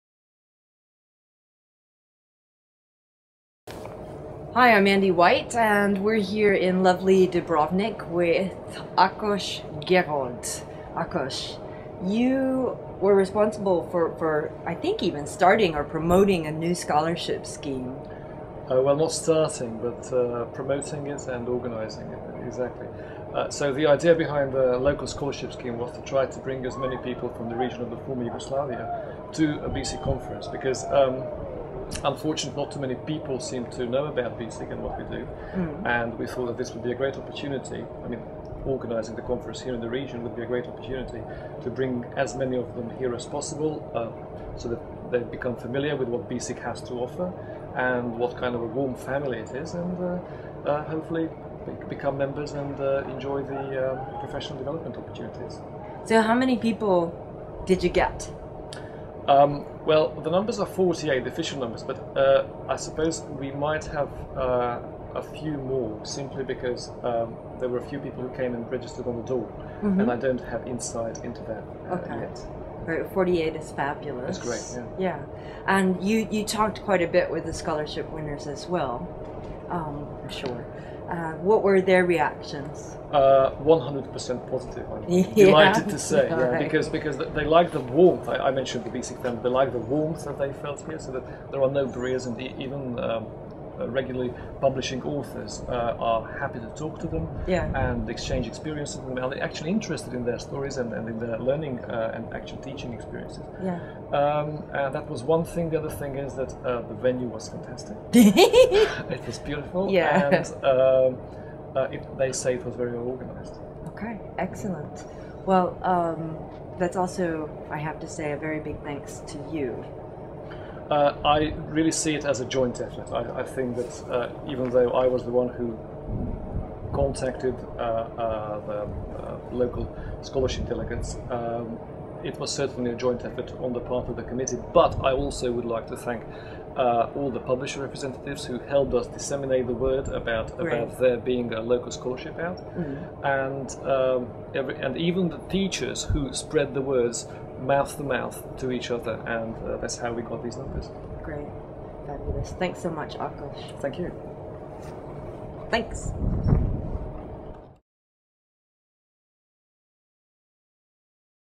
Akos Gerold
Andi White from the BESIG online team interviews Akos Gerold about BESIG's new scholarship program.